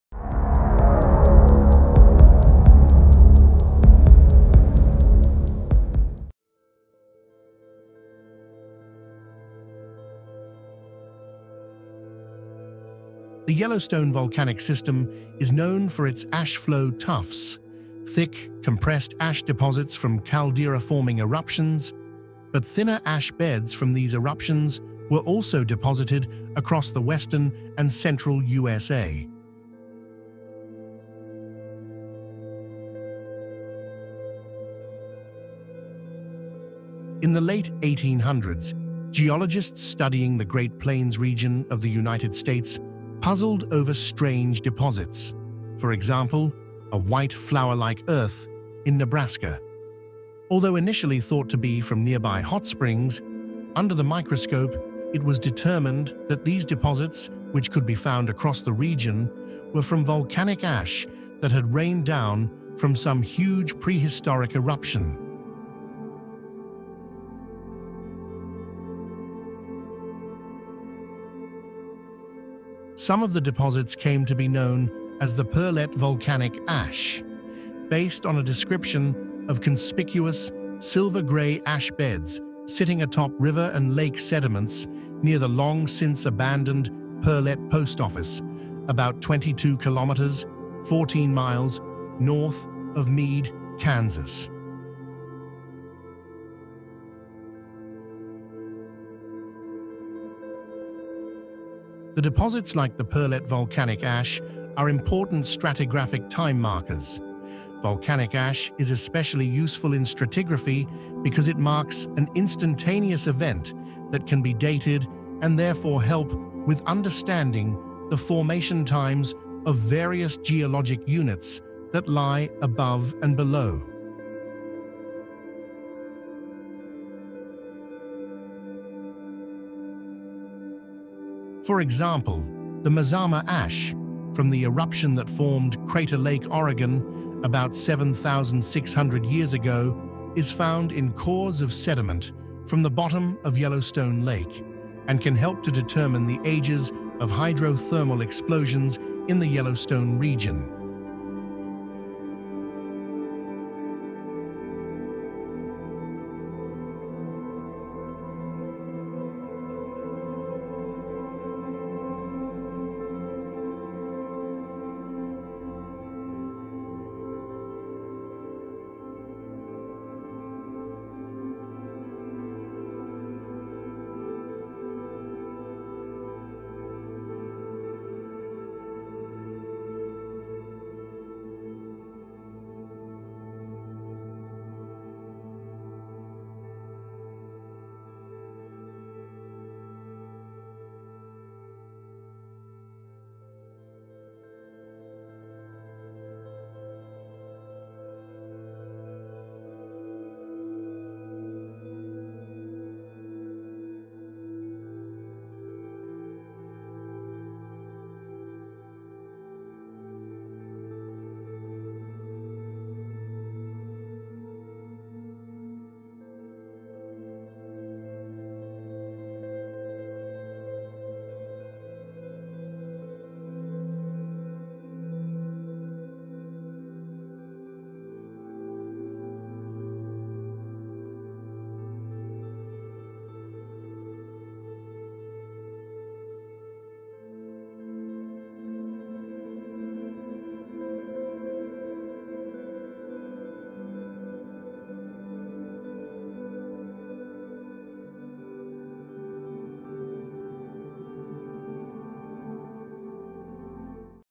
Horrible Today: Yellowstone Volcano Massive Eruption Terrifying Seconds America flying buried in ash
The Yellowstone volcanic system is known for its ash-flow tuffs—thick, compressed ash deposits from caldera-forming eruptions. But thinner ash beds from these eruptions were also deposited across the western and central USA.

In the late 1800s, geologists studying the Great Plains region of the United States puzzled over strange deposits.  For example, a “white, flour-like earth” in Nebraska. Although initially thought to be from nearby hot springs, under the microscope it was determined that these deposits, which could be found across the region, were from volcanic ash that had rained down from some huge prehistoric eruption.

Some of the deposits came to be known as the “Pearlette volcanic ash” based on a description of conspicuous, silver-gray ash beds sitting atop river and lake sediments near the long-since abandoned Pearlette post office, about 22 kilometers (14 miles) north of Meade, Kansas.